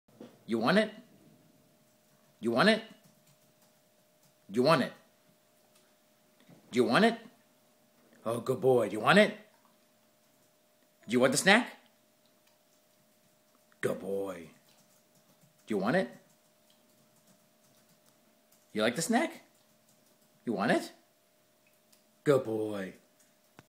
Guinness the Greater Swiss Mountain Dog. He licks his chops when you ask if he wants a snack.